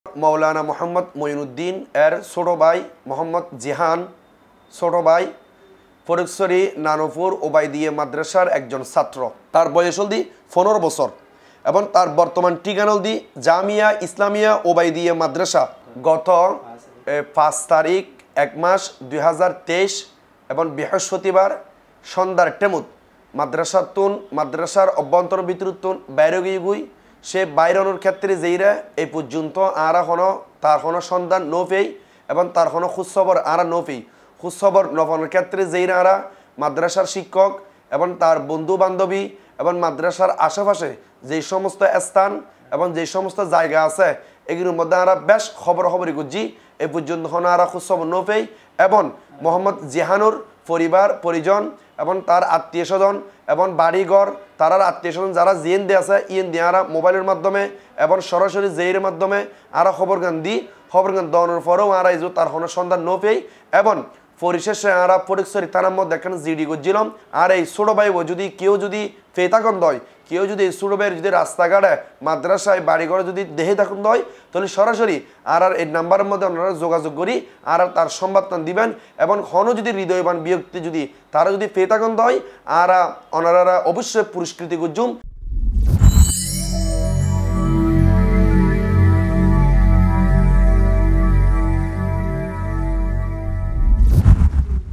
জিহানকে খুঁজে পেতে সবার সাহায্য চায় পরিবার , যোগাযোগঃ ০১৮৬৪-১৯৫৯৩২ | Cplus
জিহানকে খুঁজে পেতে সবার সাহায্য চায় পরিবার , যোগাযোগঃ ০১৮৬৪-১৯৫৯৩২

Slogan: আরাঁ চাটগাঁইয়া হতা হই
Country: Bangladesh
Language: Bengali।Chittagong Language
Broadcast area: Online